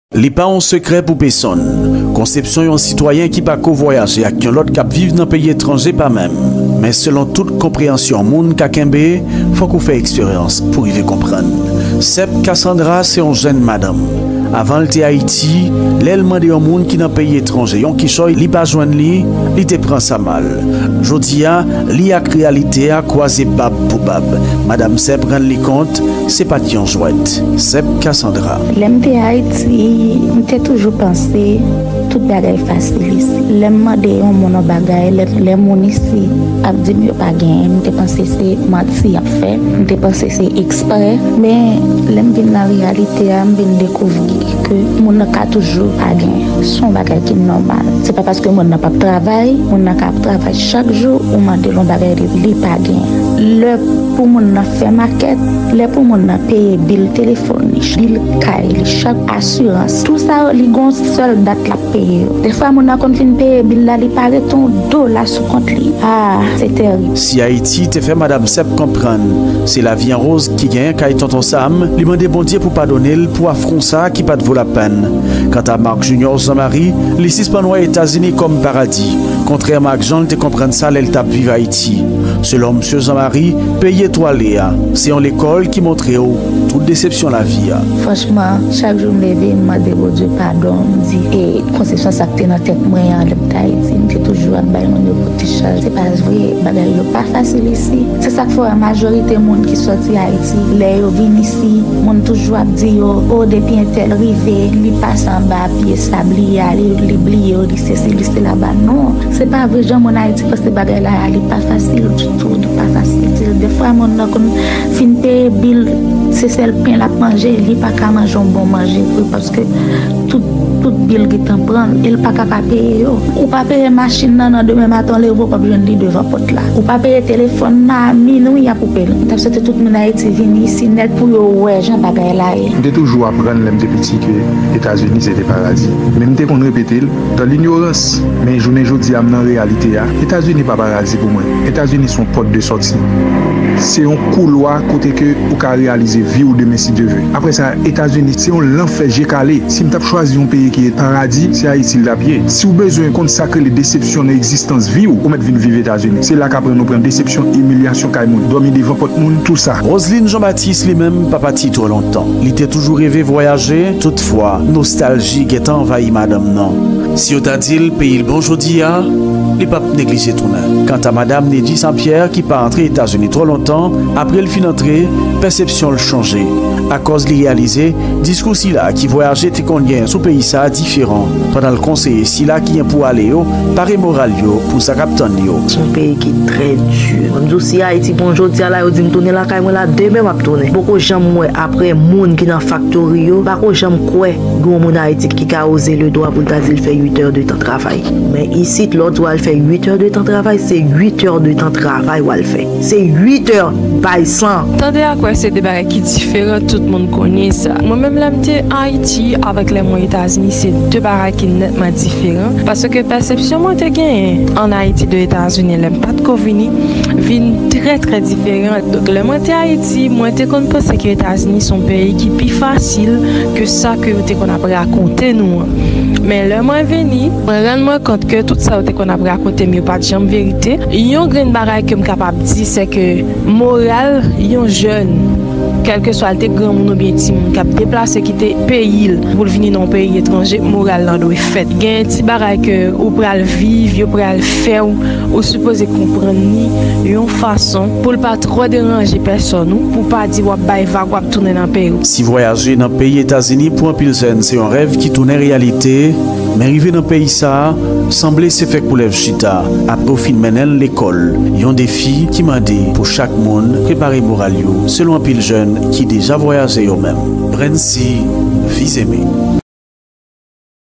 SIW GEN POU ALE ETAZINI GADE VIDEO SA AVAN😥 ESKE ETAZINI SE PARADI VRE??? Brincy Fils-Aimé#
Reportage et voix: Brincy Fils Aimé

apre video sa wap gen yon lòt pèsepsyon de moun ou kap viv nan USA.